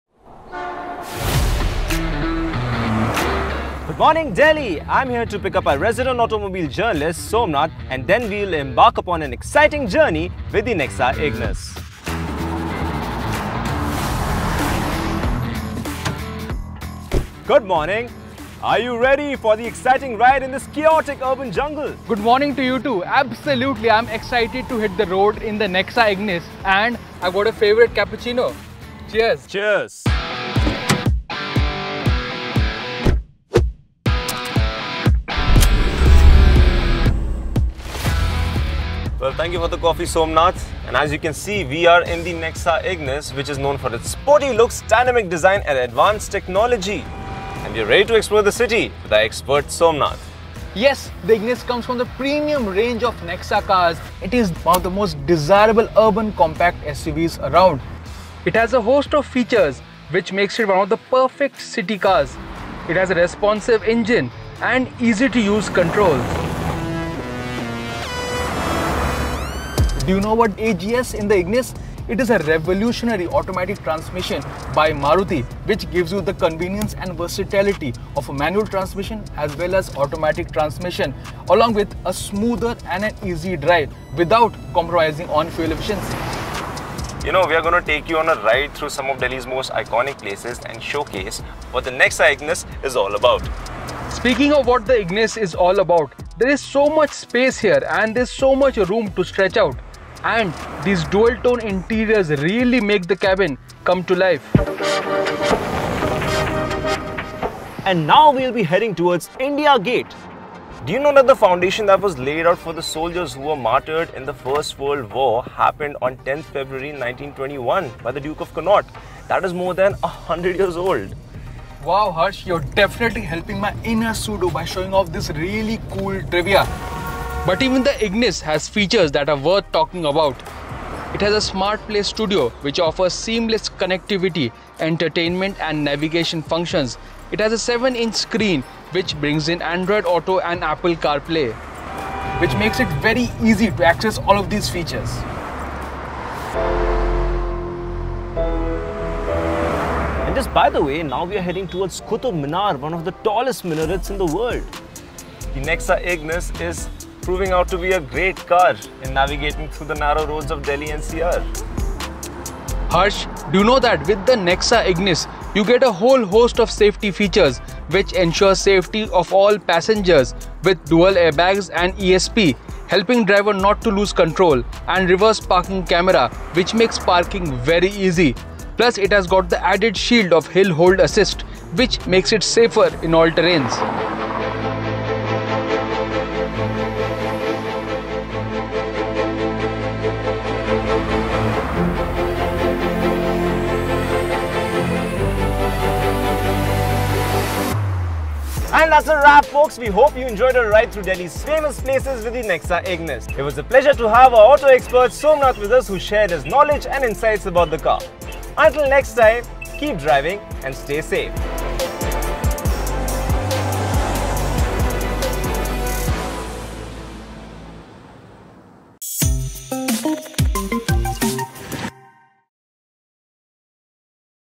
Exploring Delhi in the Tough Urban NEXA Ignis
Welcome to our exciting adventure in Delhi! Join us on a captivating journey as we explore this incredible city's vibrant streets and iconic landmarks, all from the comfort of our NEXA Ignis, the perfect city car. Get ready to be mesmerized by the rich history and diverse culture that Delhi has to offer.

Sit back, relax, and let the Tough Urban Ignis take you on an unforgettable tour of Delhi.